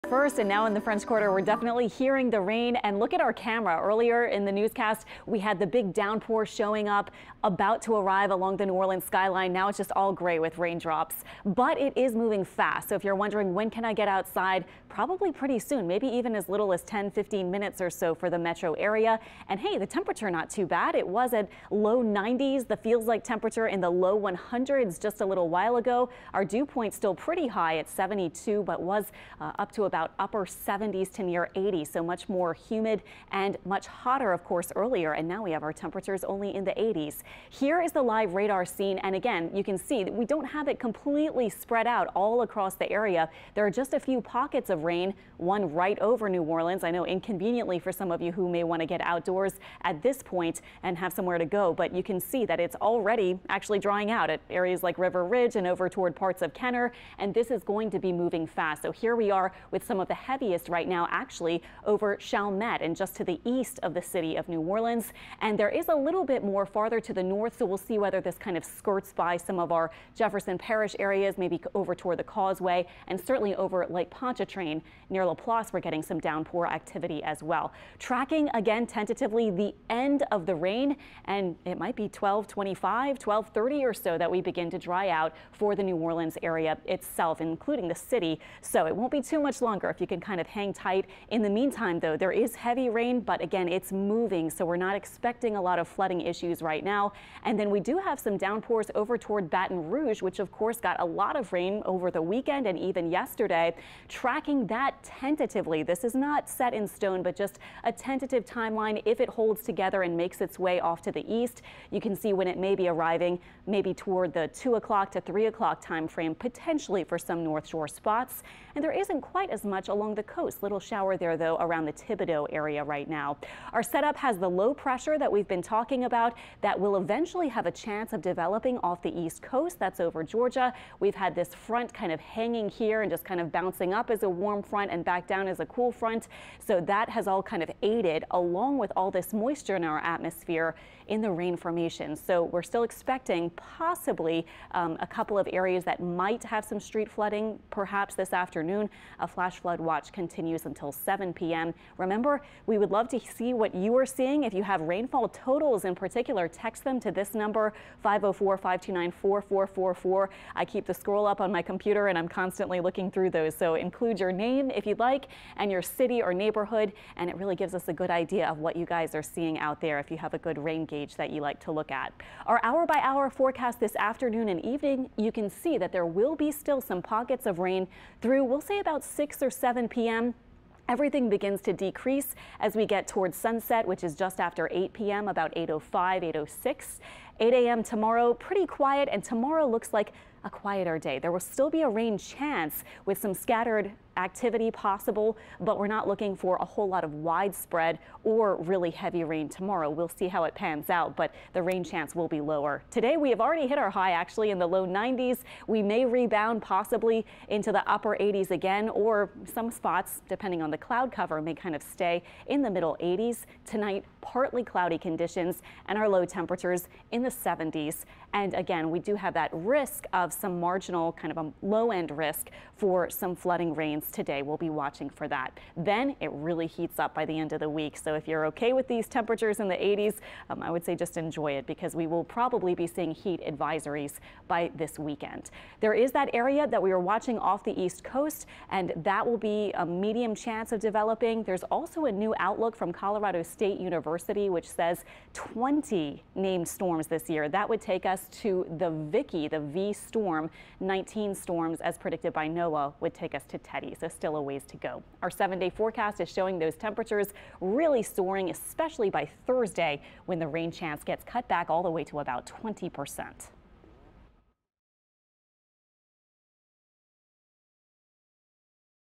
Weather: Tracking storms on Tuesday afternoon
Meteorologist Alexandra Cranford has the forecast at noon on Tuesday, July 7, 2020.